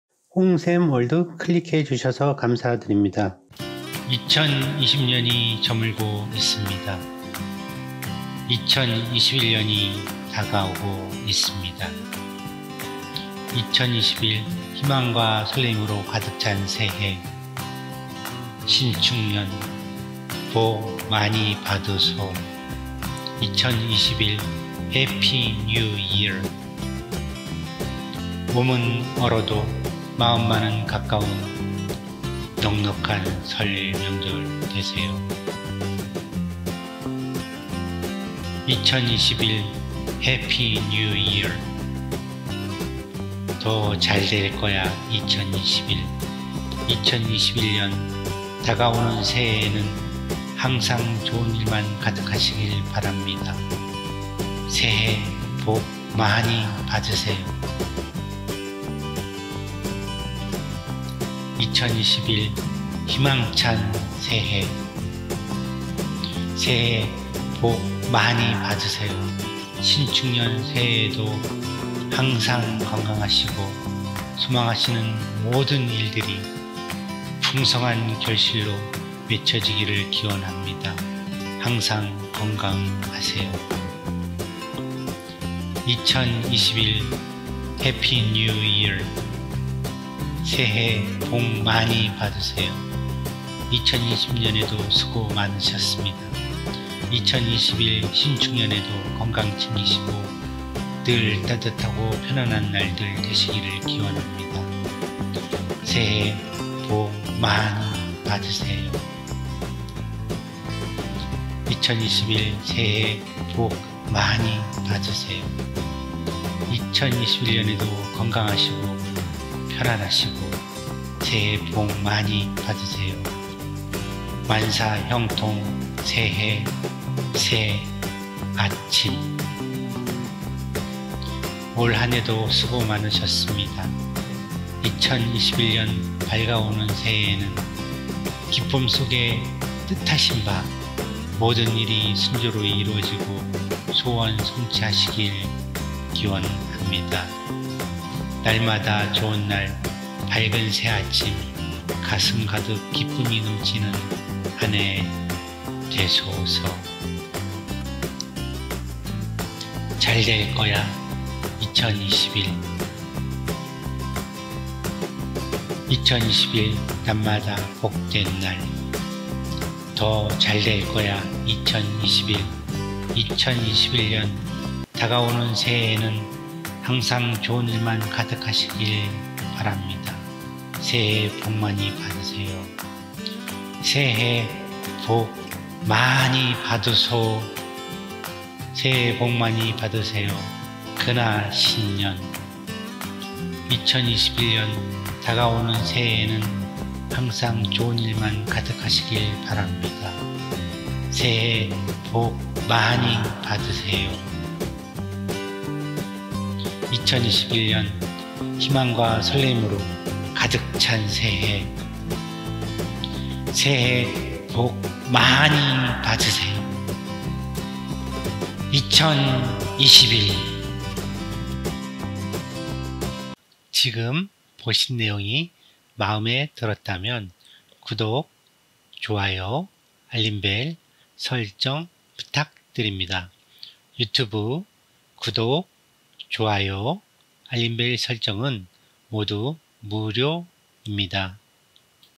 2021신축년 신년카드, 새해 복 많이 받으세요
2021신축년 신년카드, 새해 복 많이 받으세요

올 한 해도 수고 많으셨습니다.
2021년 밝아오는 새해에는
기쁨속에 뜻하신 바
모든 일이 순조로이 이루어지고
소원성취 하시길 기원합니다.

신축년 새해에도 항상 건강하시고
소망하시는 모든 일들이
풍성한 결실로 맺혀지기를 기원합니다.